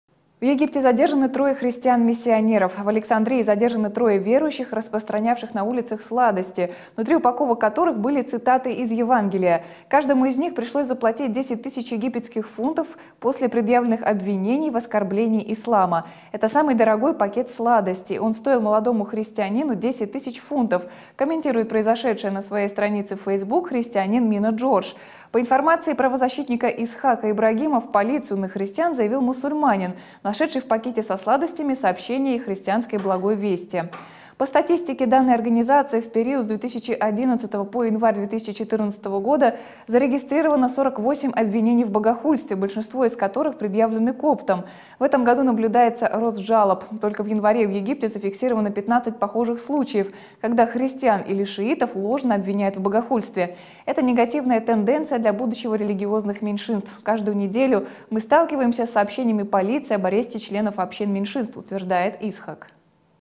В Египте задержаны трое христиан-миссионеров
В Александрии задержаны 3 верующих, распространявших на улицах сладости, внутри упаковок которых были цитаты из Евангелия. Каждому из них пришлось заплатить 10 тыс. египетских фунтов после предъявленных обвинений в «оскорблении ислама».
«Это самый дорогой пакет сладостей. Он стоил молодому христианину 10 тыс. фунтов», - комментирует происшедшее на своей странице Фейсбук христианин Мина Джордж.
По информации правозащитника Исхака Ибрагима, в полицию на христиан заявил мусульманин, нашедший в пакете со сладостями сообщение христианской Благой Вести.
По статистике данной организации, в период с 2011 по январь 2014 г. зарегистрировано 48 обвинений в «богохульстве», большинство из которых предъявлены коптам. В этом году наблюдается рост жалоб. Только в январе в Египте зафиксировано 15 похожих случаев, когда христиан или шиитов ложно обвиняют в «богохульстве».
«Это негативная тенденция для будущего религиозных меньшинств. Каждую неделю мы сталкиваемся с сообщениями полиции об аресте членов общин меньшинств», - утверждает Исхак.